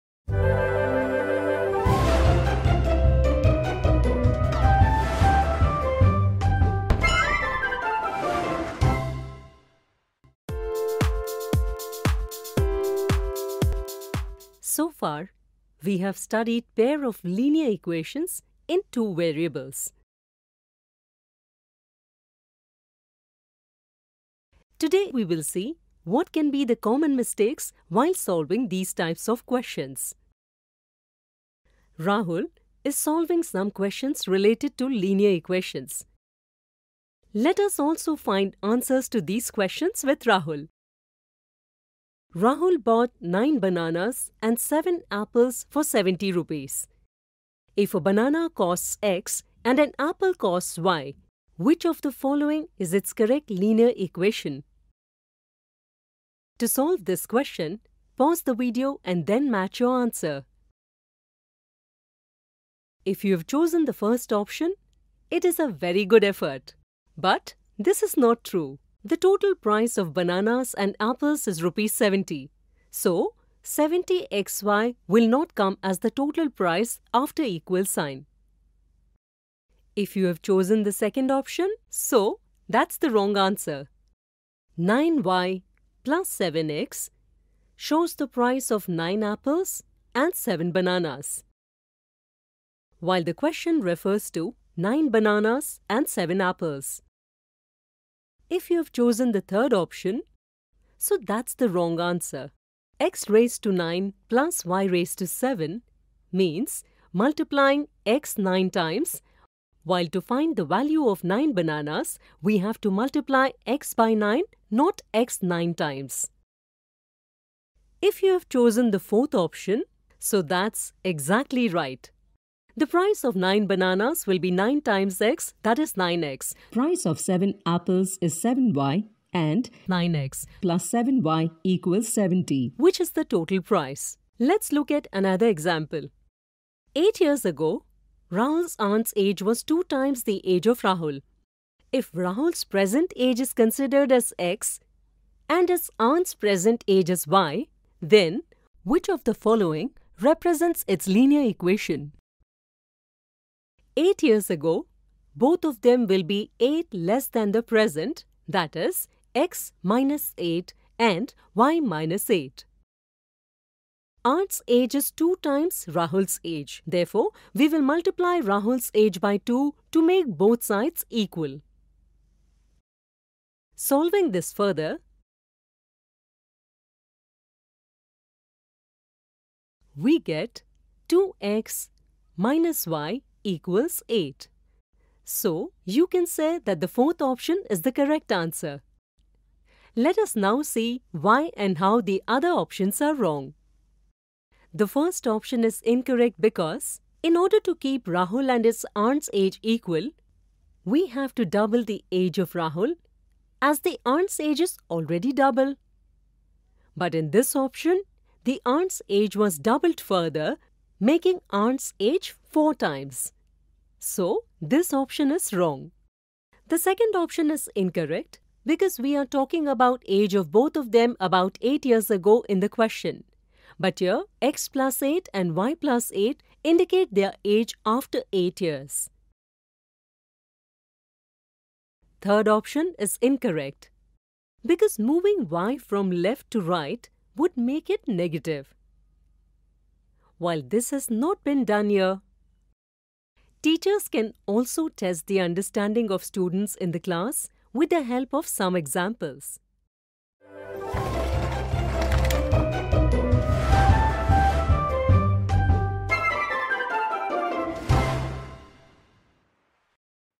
Pair of linear equations in two variables | Part 3/3 | English | Class 10
Pair of linear equations in two variables | Part 3/3 | English | Class 10 | NCERT

Pair of linear equations in two variables | Linear Equations in Two Variables | Grade 10 | NCERT | Pair of Linear equations in two variables (4)

Welcome to part 3 of Pair of linear equations in two variables. In this video, we will discuss various misconceptions and/or common errors related to Pair of linear equations in two variables.